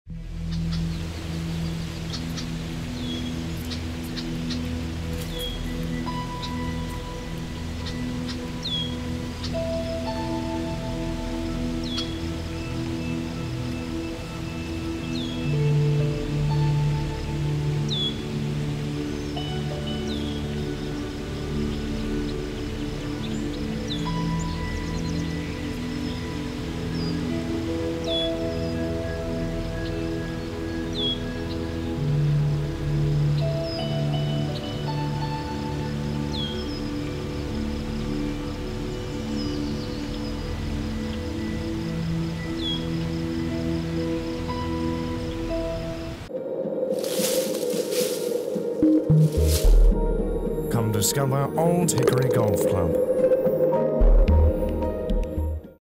Old Hickory Golf Club Hole #18
Course Designer, P.B. Dye.
Hole #18 which is a Par 4.
Old Hickory Golf Club in St. Peters, MO